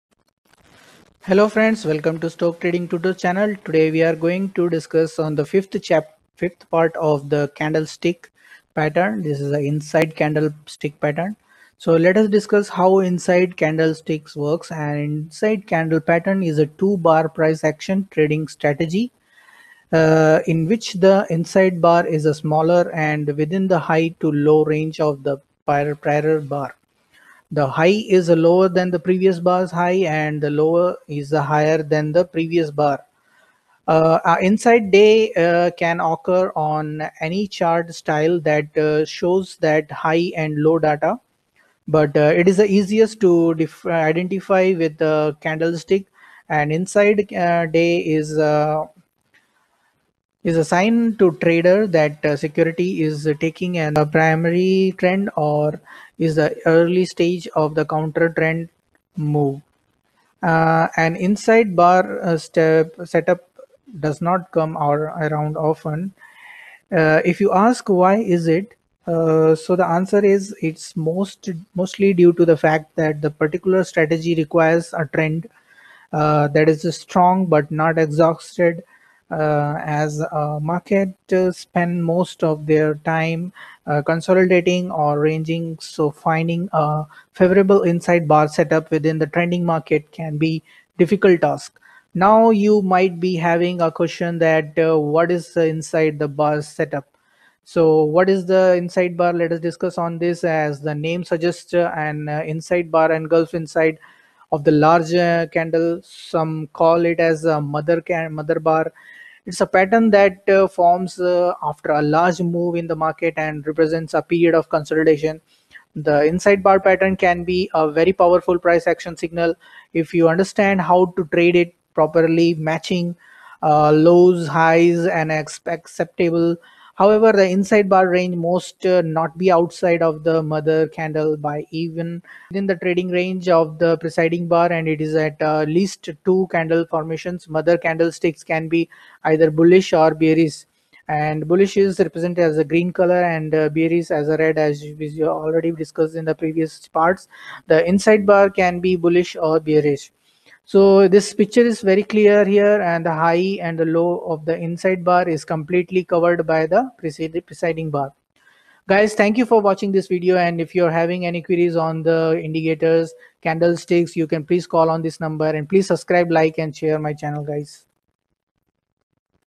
Inside Candle Stick Candle Sticks Pattern Part 5 } STT
An inside bar setup doesn’t come around often. If you ask, why is it? So the answer is, it’s mostly due to the fact that this particular strategy requires a trend that is strong but not exhausted.